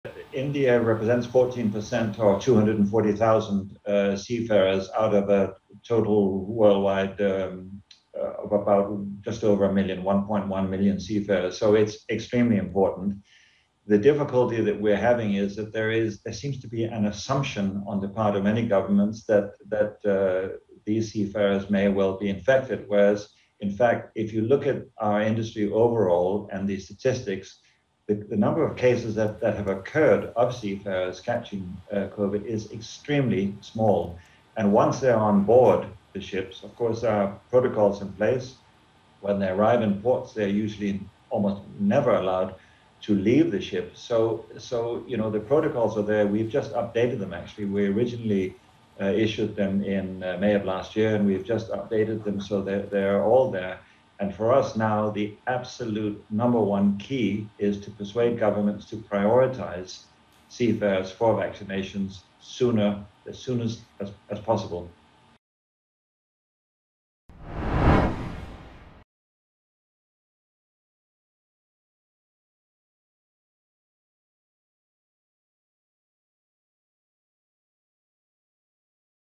Governments should prioritize seafarers for Covid vaccines, says shipping executive
Esben Poulsson, chairman of the International Chamber of Shipping, says seafarers already face tight port restrictions and constitute a small fraction of cases.